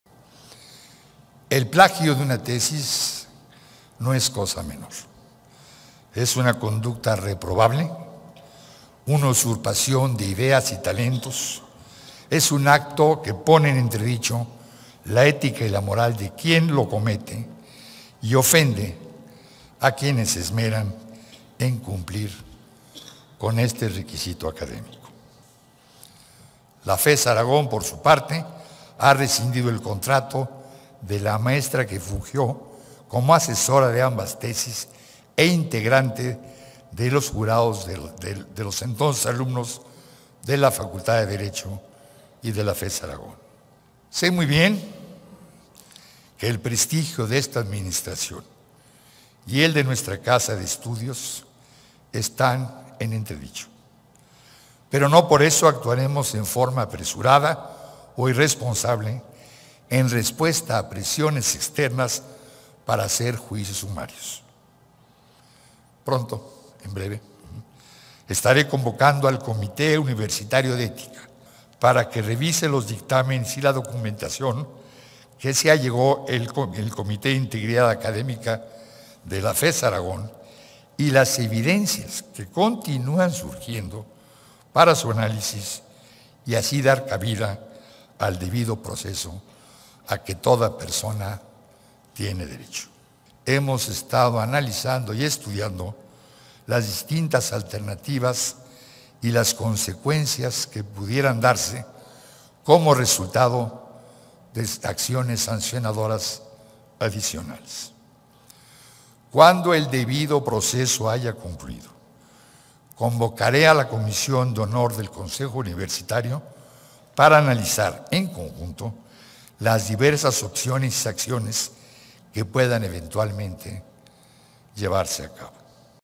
UNAM convocará a Comité Universitario para analizar caso Yasmín Esquivel
Enrique Graue, rector de la Universidad Nacional Autónoma de México dio a conocer a través de un mensaje sobre el tema de las tesis que ha ocupado la atención de la comunidad universitaria, de los medios y del público en general.